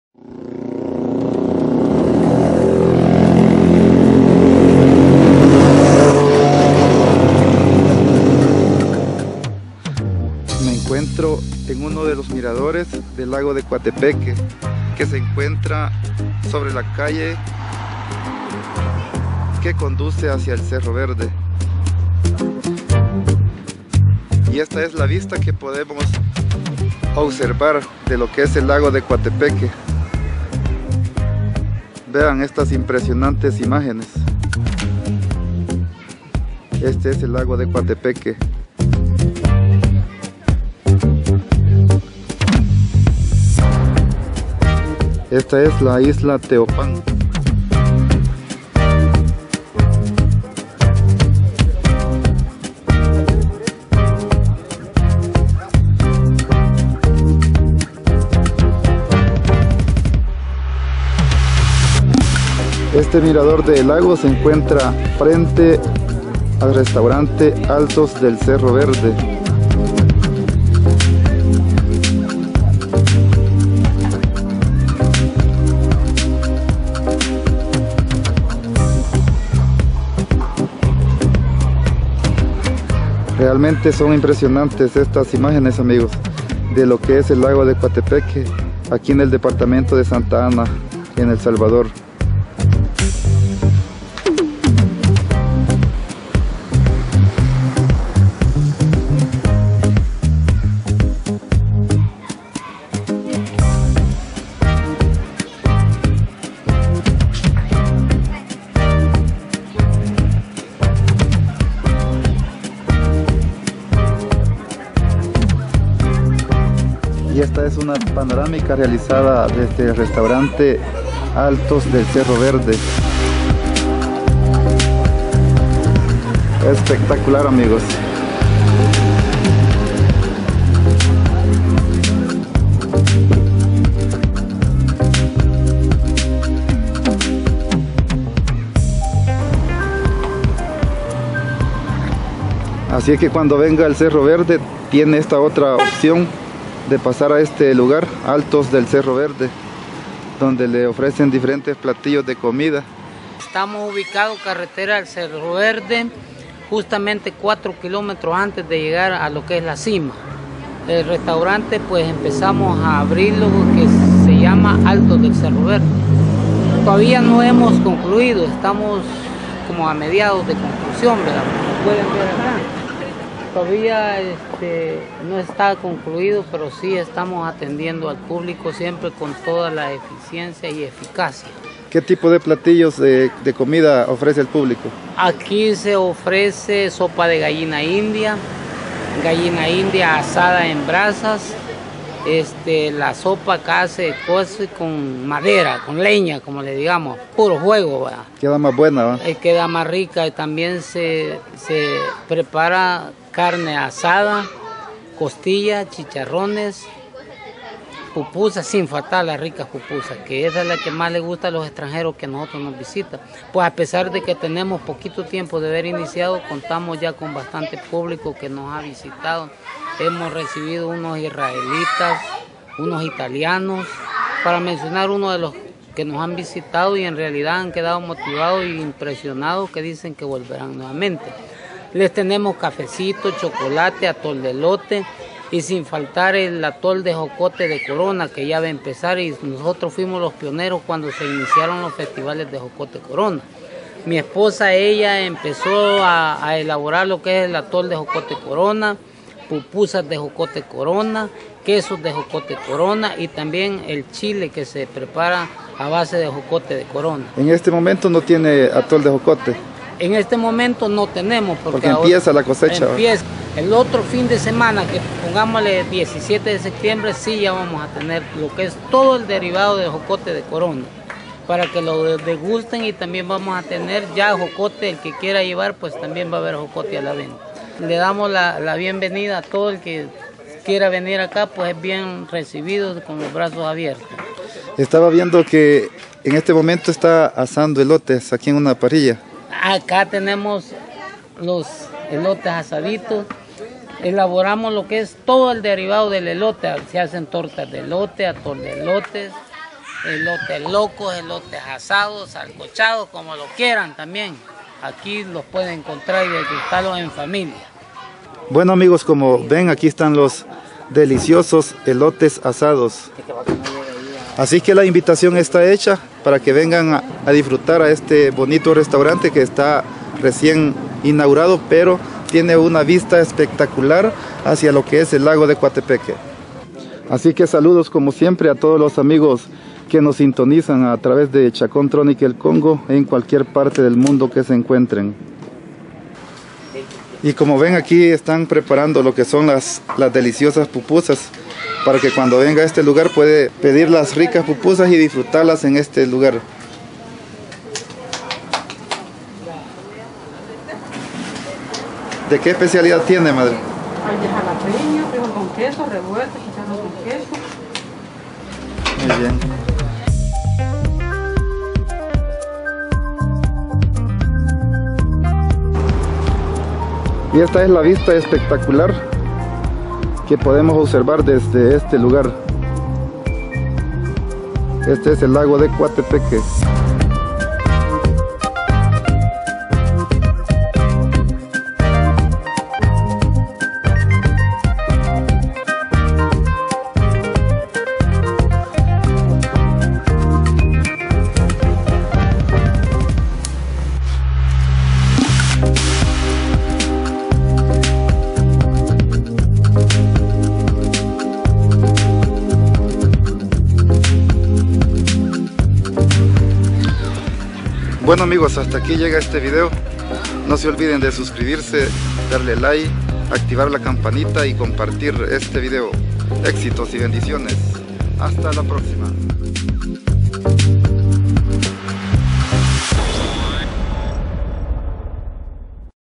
Restaurante "Altos del Cerro Verde" Santa Ana. El Salvador.
Un excelente lugar donde puedes ir a disfrutar de un rico plato de comida y con una vista espectacular hacia el Lago de Coatepeque.